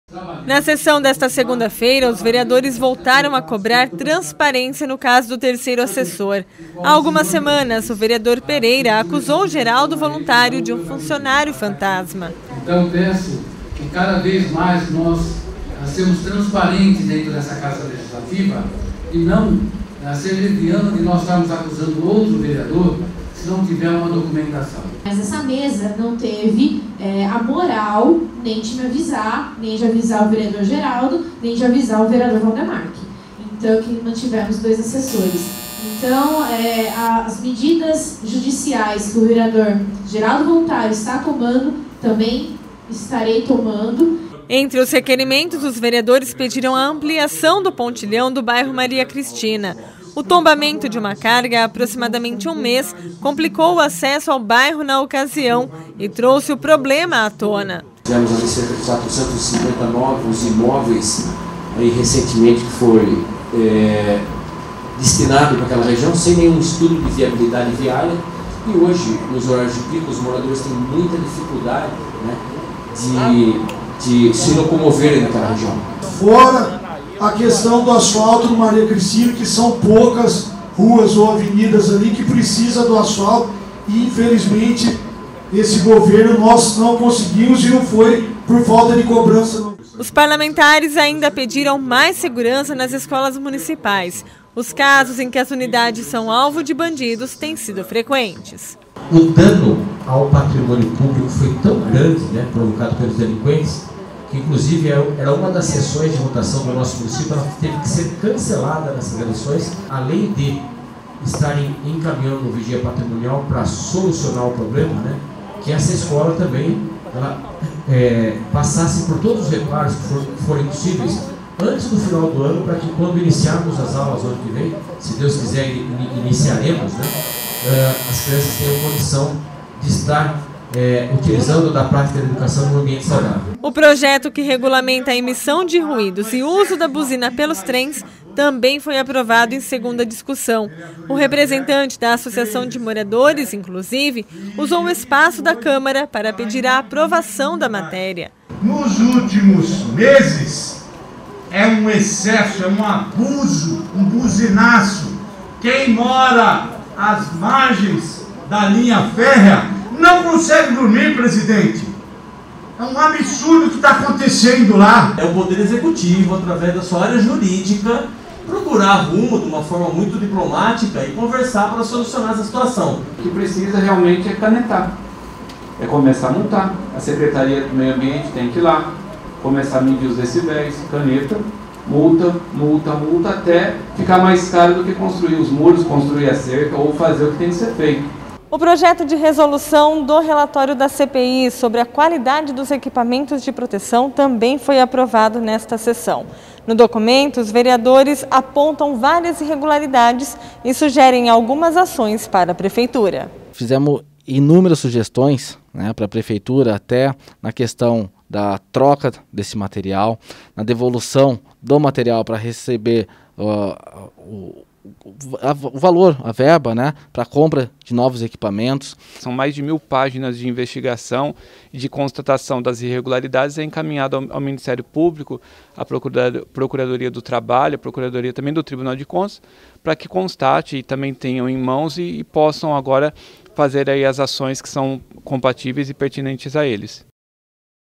Investigação dos EPI's comprados por R$ 4 mi voltam à pauta da Câmara
Projeto de resolução do relatório da CPI foi aprovado pelos vereadores.